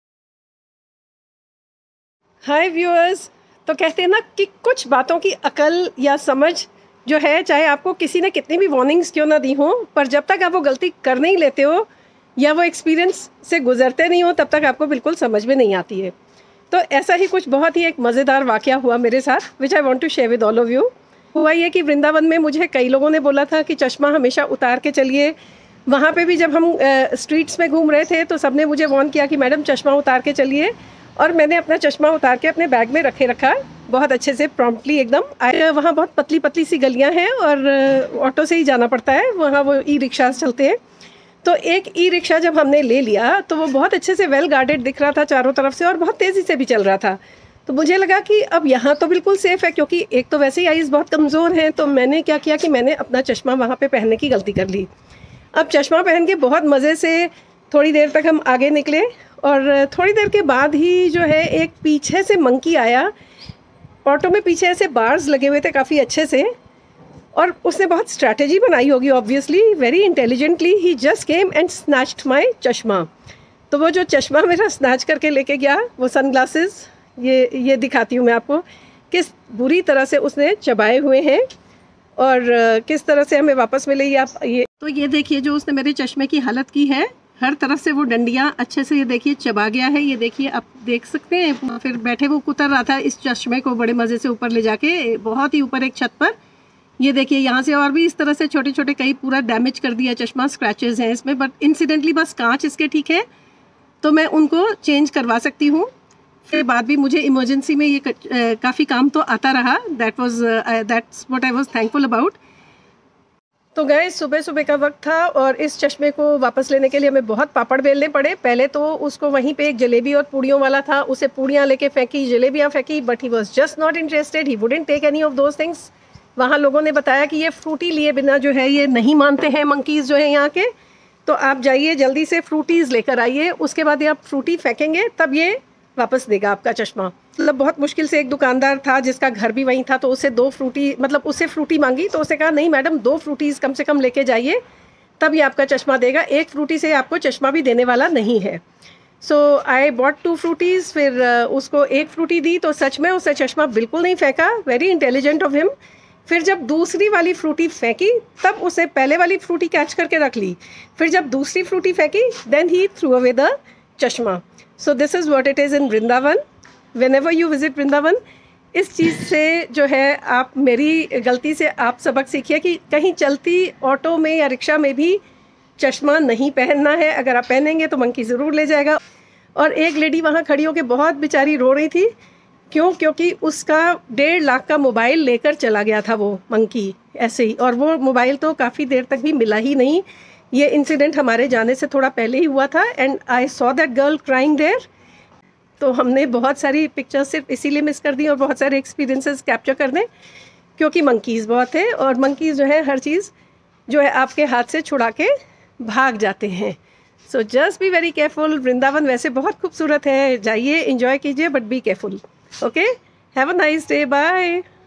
बंदर मेरी ऐनक लेकर भाग गया 😭🐒🙊traveldiaries
How a monkey ran away with my specks and what strategy we had to apply to get it back @vrindavanfiles